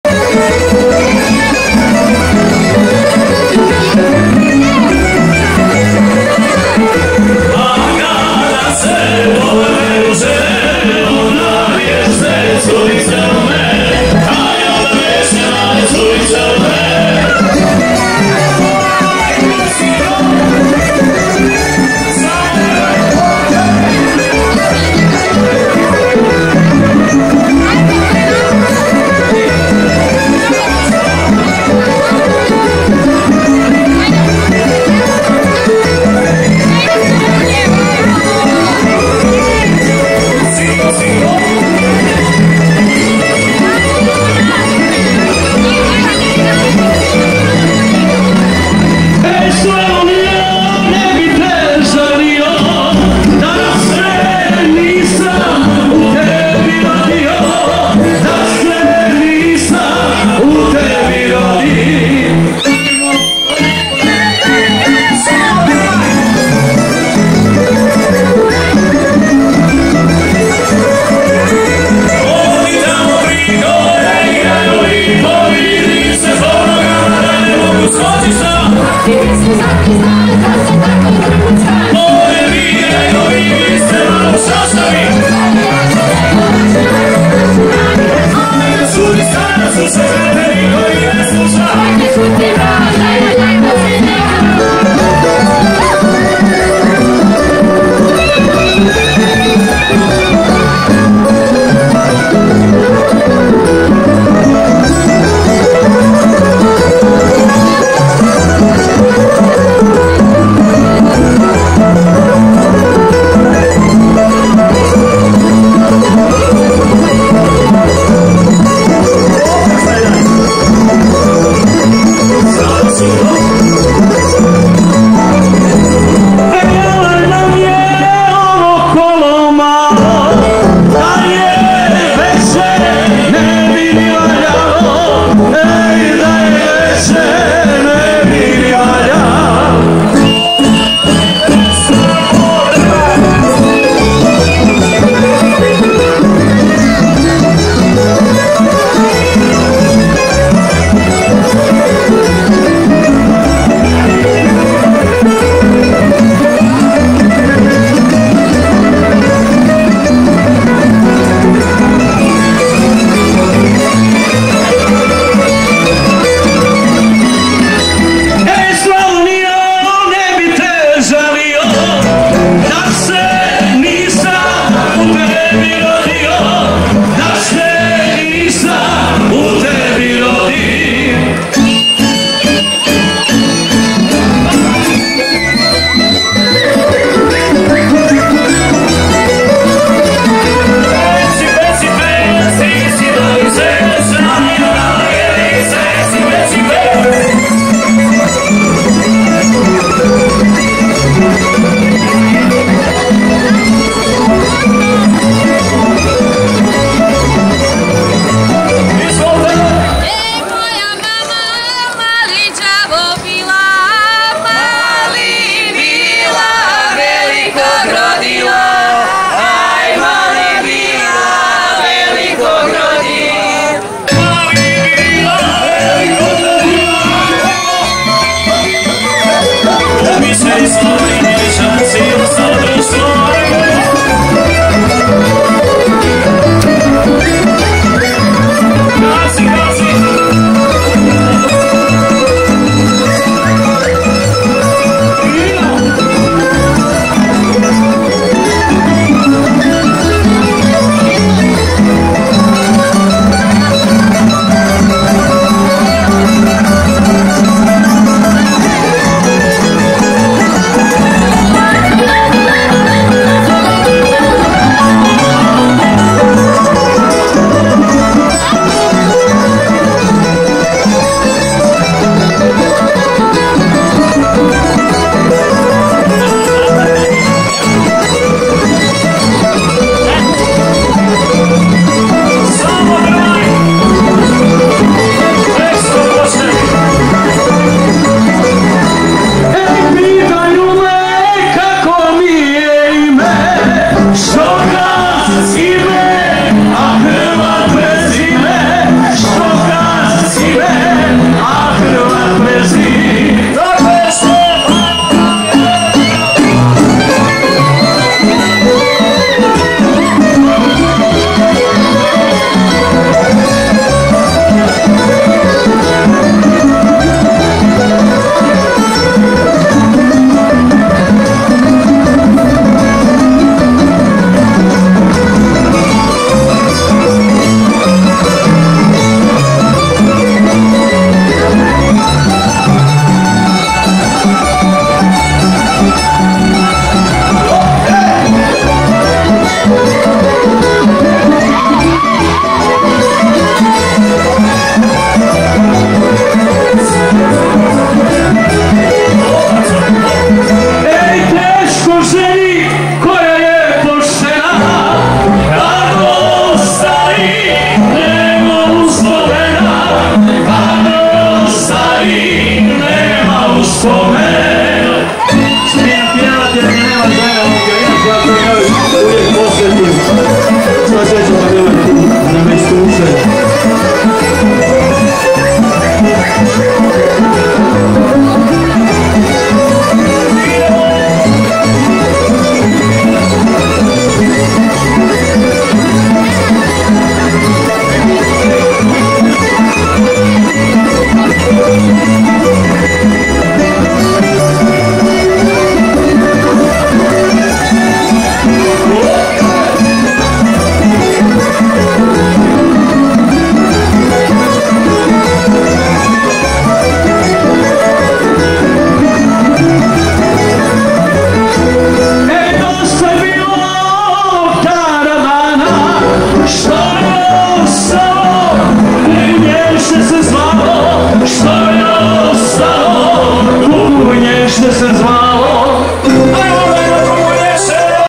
49. SMOTRA FOLKLORA MIHOLJAČKO SIJELO I JERŠEK ŠTEF
U trećem  danu  održavanja 49. smotre folklora Miholjačko sijelo  u dvorištu dvorca Prendau Majlathu u Donjem Miholjcu,  uz prateći zvuk tamburica, mještane i goste zabaviio je Jeršek Štef.